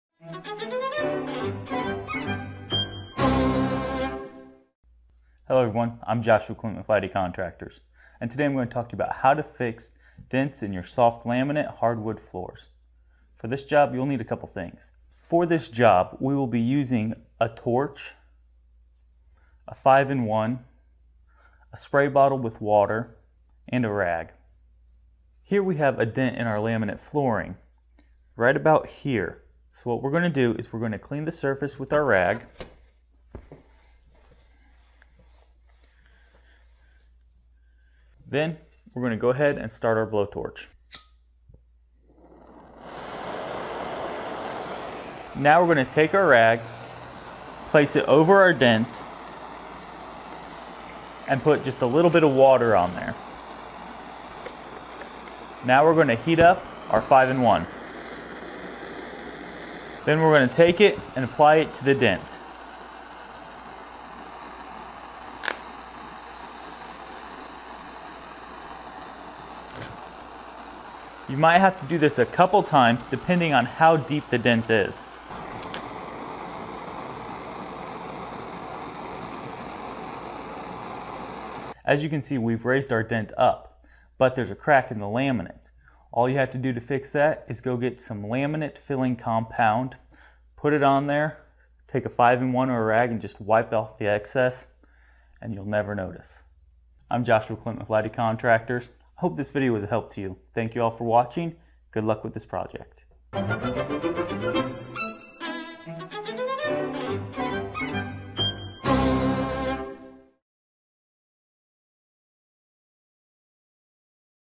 How Do I Fix Soft Laminate Hardwood Floor Dents?
How Do I Fix Soft Laminate Hardwood Floor Dents?. Part of the series: Let's Talk Flooring. You can fix a soft laminate hardwood floor in a variety of different ways depending on the specific nature of the problem that you're having. Fix soft laminate hardwood floor with help from a foreman for Lighty Contractors in this free video clip.